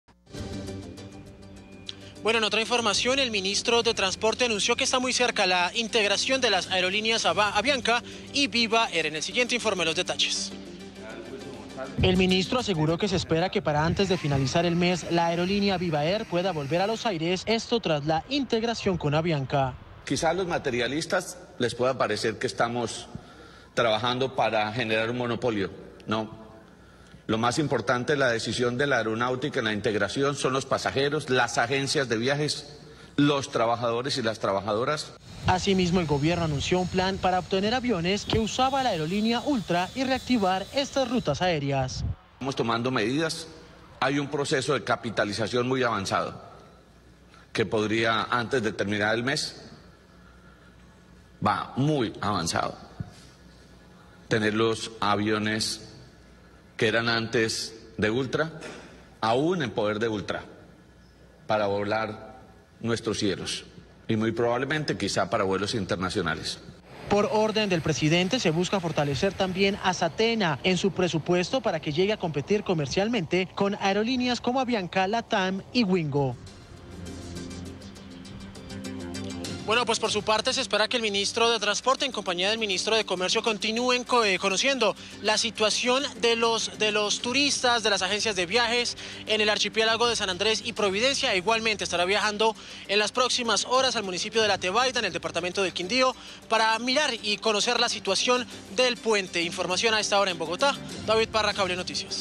Se ultiman detalles para la integración entre Viva Air y Avianca
El ministro de Transporte, Guillermo Reyes, aseguró que se espera que la integración entre Avianca y Viva Air pueda estar lista antes de terminar el mes de abril.